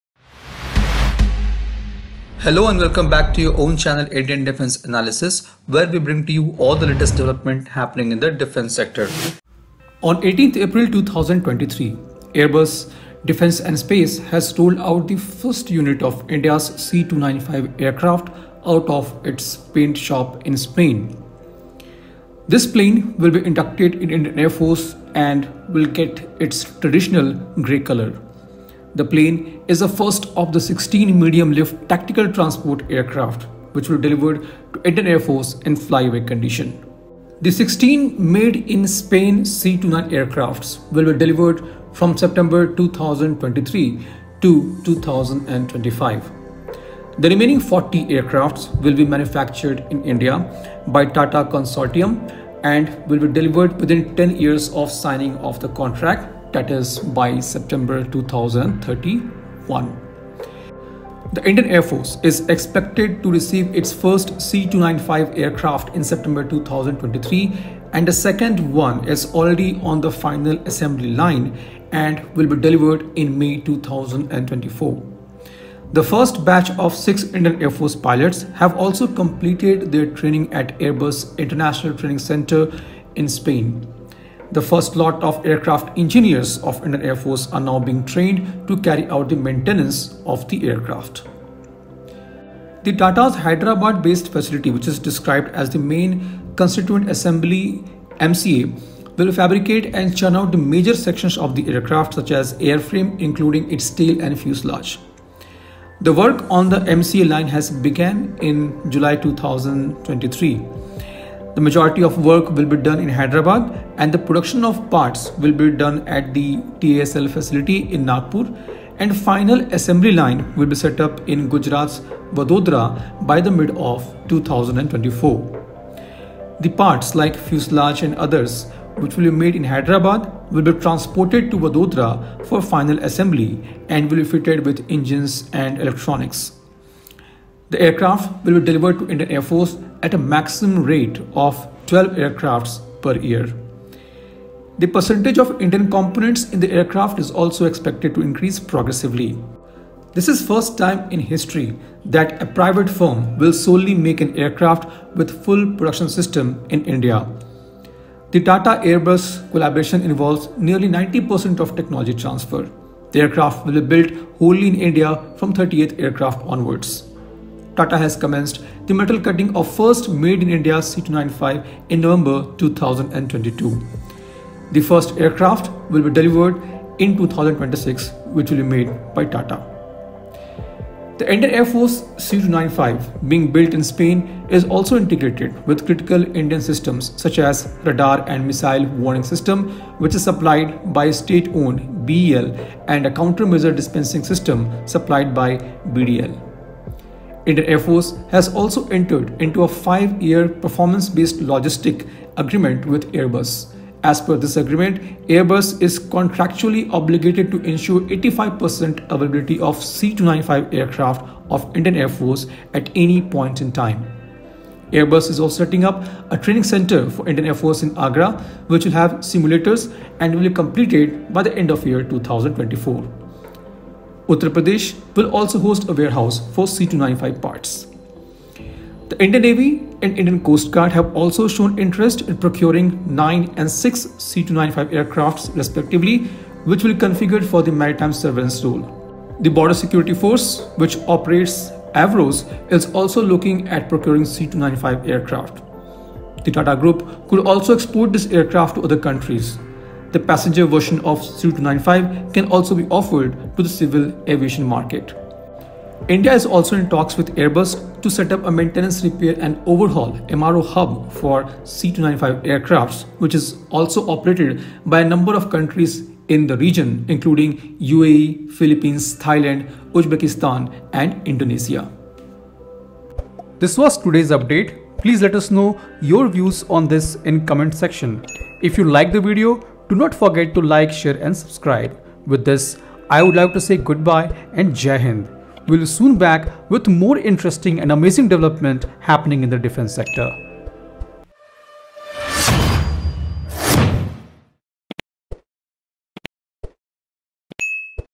breakingnews India begins manufacturing C 295 aircraft, 15 more for Navy & ICG
IAF C295 aircraft delivery

On 18th April 2023, Airbus Defence and Space has rolled out the first unit of India’s C-295 aircraft out of its paint shop, at the production facility in the city of Seville, located in southern Spain. The plane will be inducted in the Indian Air Force and gets their traditional grey colour. The plane is the first of the 16 medium-lift tactical transport airport aircraft which will be delivered to the Indian Air Force (IAF) in flyaway condition.

These aircrafts will be delivered between September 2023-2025. The remaining 40 aircrafts will be manufactured in India by TATA Consortium & delivered within 10 years of signing of the contract i.e., by September 2031.

IAF 2nd C295 in assembly line in Spain

Indian Air Force is expected to receive first C295 in September 2023 and the second is already on the final assembly line and will be delivered in May 2024. The first batch of 6 IAF pilots have completed their training at Airbus International Training Centre Seville, Spain. The first lot of aircraft engineers of the IAF are now being trained to carry out maintenance of the aircraft.

TASL Manufacturing facilities

Tata’s Hyderabad facility described as the main constituent assembly (MCA) will fabricate and churn out major sections of the aircraft such as airframe, including its tail and fuselage.

Work on the Main Constituent Assembly (MCA) Line has begun in July 2023. The majority of the work will be done in Hyderabad and production of parts will be done at the TASL facility in Nagpur & the final assembly line will be set up in Gujarat’s Vadodara by mid of 2024.

The parts like fuselage and others made in Hyderabad will be transported to Vadodara for final assembly and will be fitted with engines and electronics. The aircrafts will be delivered to the IAF at a maximum rate of a dozen aircraft per year. The percentage of Indian component in the aircraft is expected to increase progressively.

This is the first time in history that an Indian private firm will solely make an aircraft with full production system in India. The TATA-Airbus collaboration involves, nearly 90 percent of technology transfer. The aircraft will be built whole in India from the 30th aircraft onwards.

Tata has commenced the metal cutting of 1st made-In-India C295 in November 2022. The first India-made C295 aircraft will be delivered in 2026.

The IAF C295s being built in Spain also integrated with critical Indian systems such as radar and missile warning systems supplied by the state-owned Bharat Electronics Ltd (BEL) and a counter-measure dispensing system supplied by Bharat Dynamics Ltd (BDL).

MRO & Logistics

The IAF has also entered into a five-year Performance-Based Logistics (PBL) agreement with Airbus. As per the agreement, Airbus is contractually obligated to ensure 85 percent availability for the C295 aircraft fleet of IAF at any point in time.

Airbus is also setting up a training centre for the IAF in Agra which will have simulators and will be completed by the end of 2024. Uttar Pradesh will also host a warehouse for the C-295 parts.

Future Orders of C295

Indian Navy and Indian Coast Guard have also shown interest in procuring at least nine and six C295 aircraft, respectively, configured for the maritime surveillance role. BSF which also operates the Avros, is also looking at procuring C-295 aircraft. The TATA group could also export this aircraft to other countries.The passenger version of the C295 can also be offered to civil aviation.

India is also in talks with Airbus to set up a maintenance repair and overhaul (MRO) hub for this aircraft, which is operated by a number of countries in the region, including the UAE, the Philippines, Thailand, Uzbekistan and Indonesia.